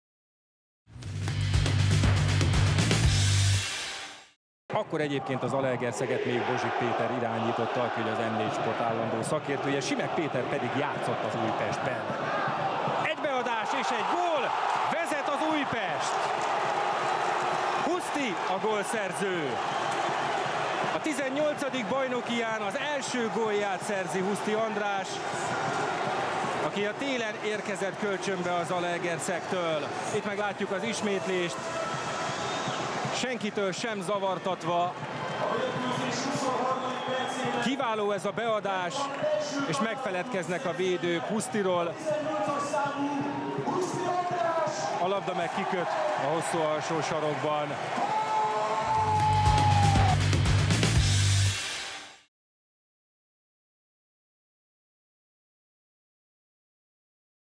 Huszti András gólja az Újpest FC - ZTE FC mérkőzésen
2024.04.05. OTP Bank Liga, 27. forduló
Újpest FC - ZTE FC | 1-5 | (1-1) |
Huszti András gólja